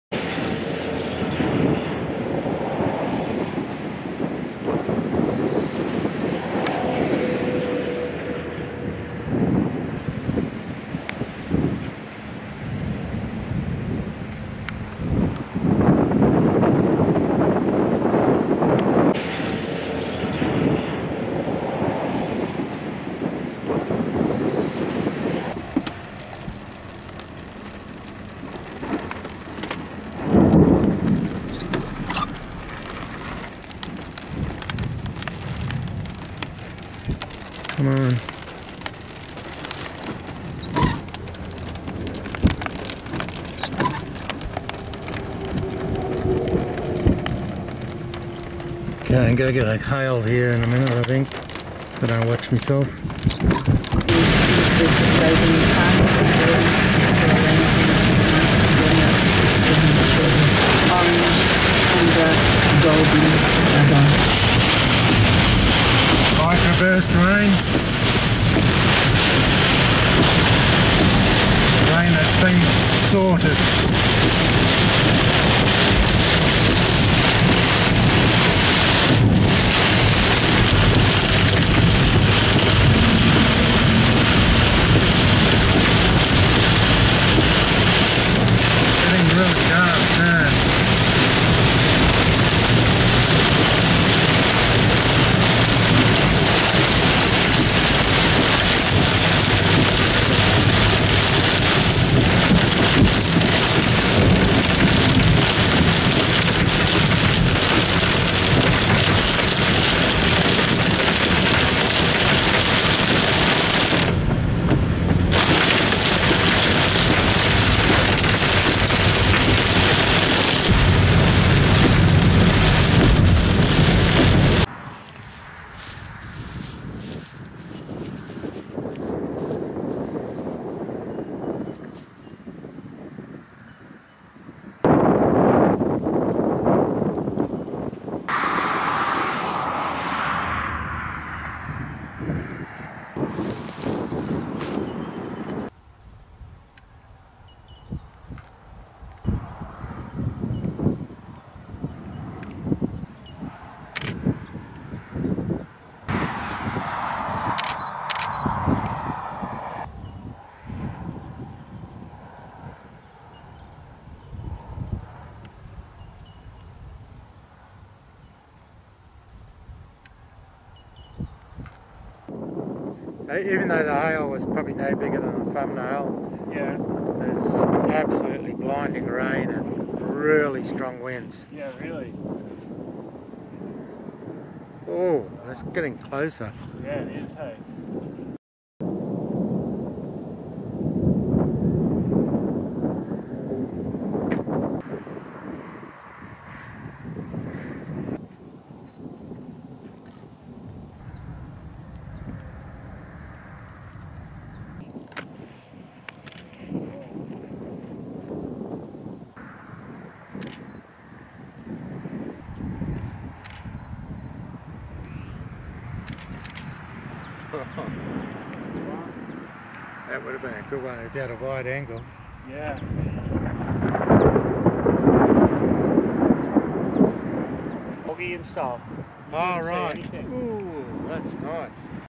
Storms - Southern Highlands and Illawarra - 3rd December 2014
Three seperate storms chased today. One was briefly severe.